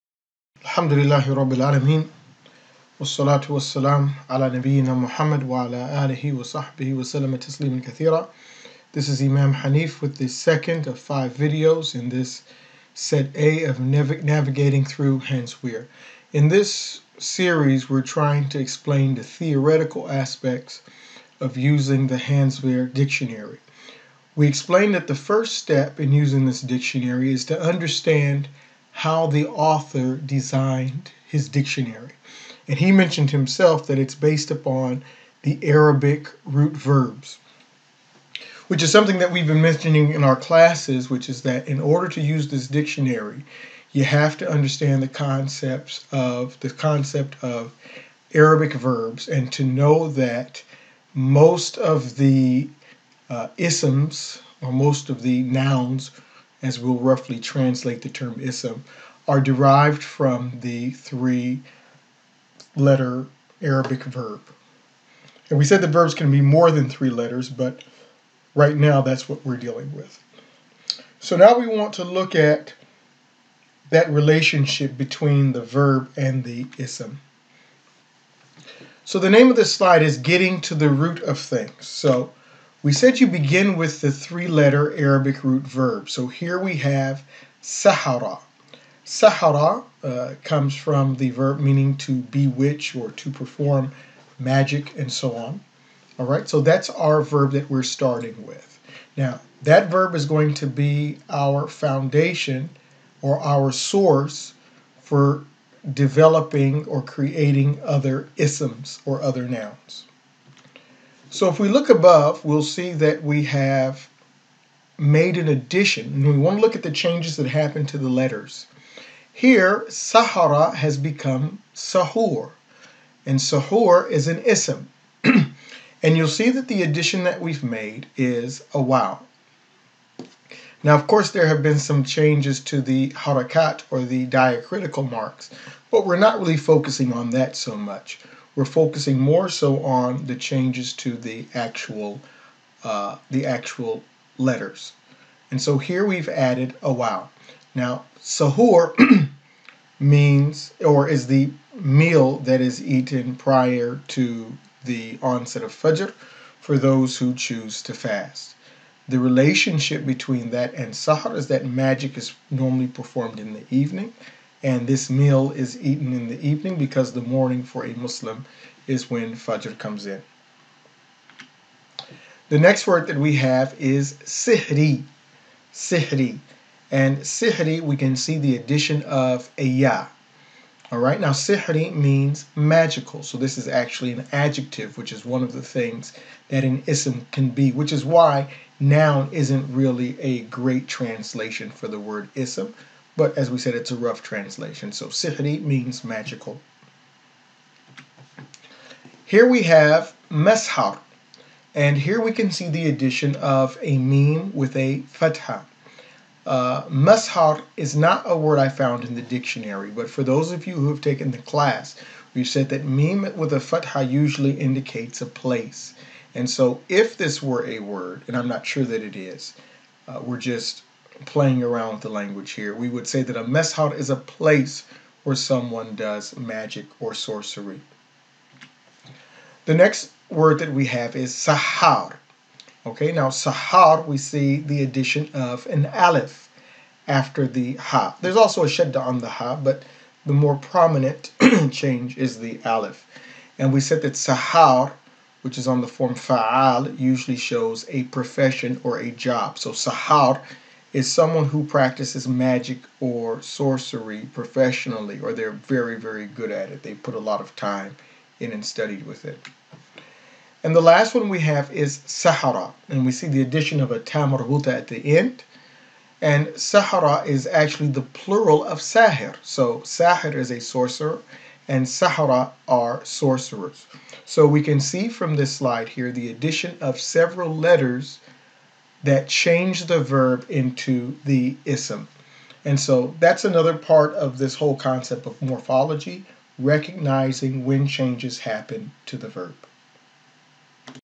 Navigating through Hans-Wehr Set A - 2 of 5
This is the second video of Set A that provides a theoretical background on how to use the Hans-Wehr Dictionary, arguably one of the best Arabic-English dictionaries available